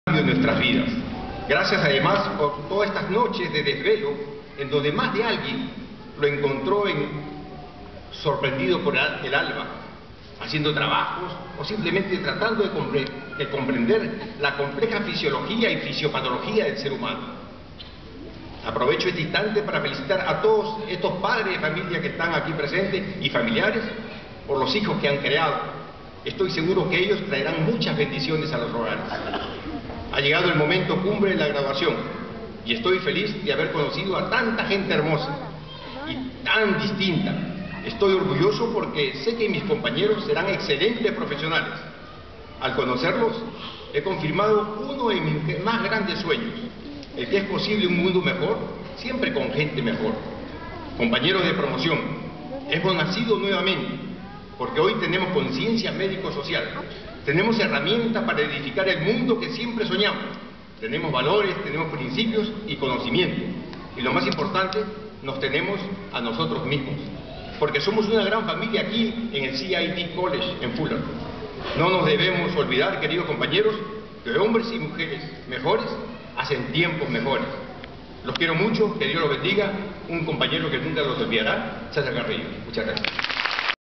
CESAR CARRILLO GRADUACION CIT COLLEGE USA [MEDICAL ASSISTANT]
GRADUACION DE CESAR CARRILLO EN EL CIT COLLEGE EN FULLERTON CALIFORNIA DE MEDICAL ASSISTANT